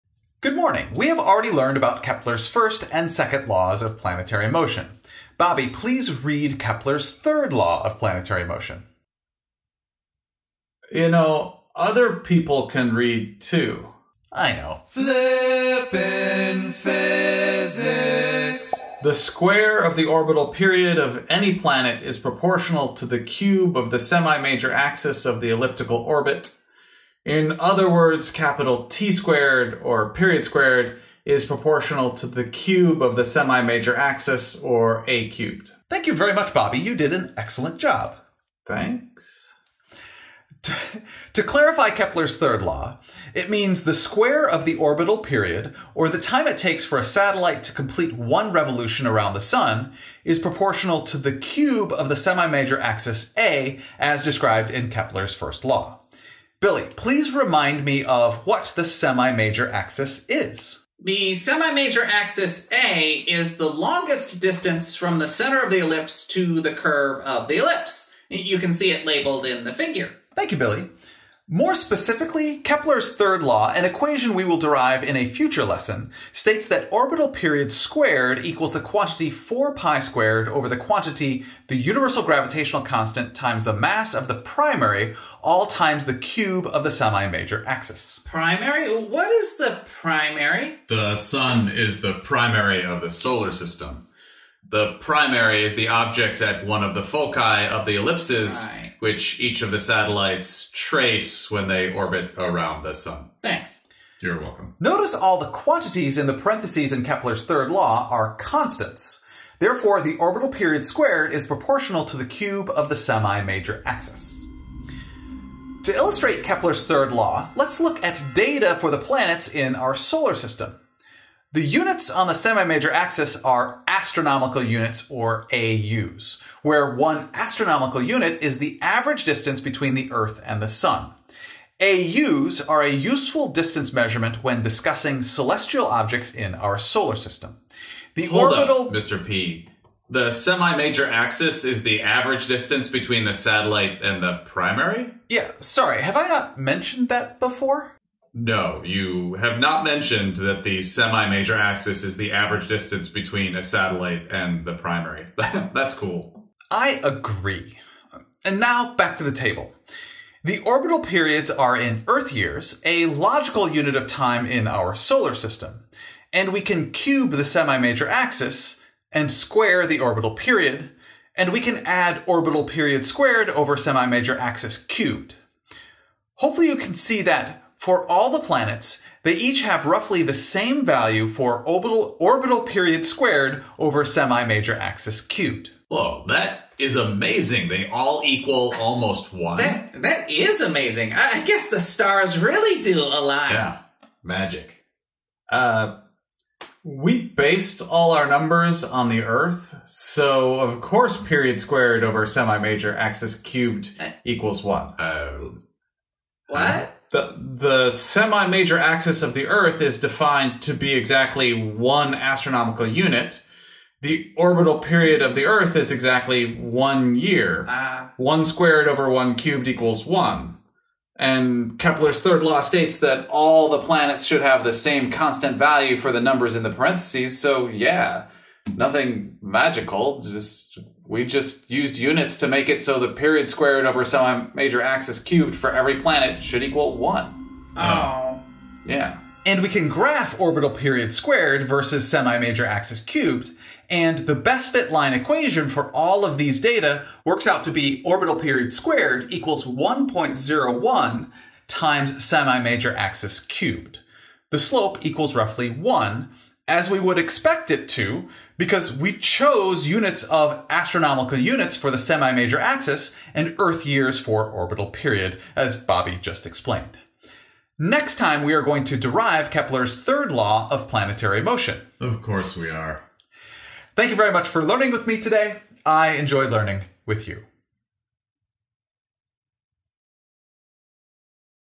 Kepler's Third Law of Planetary Motion
Kepler’s third law is described and demonstrated. The orbits of all 8 planets are compared. This is an AP Physics C: Mechanics topic.

Content Times:
0:00 Kepler’s Third Law
1:55 Comparing Planetary Data
3:58 Graphing Planetary Data

Thank you to Julie Langenbruner and John Paul Nichols being my Quality Control Team for this video.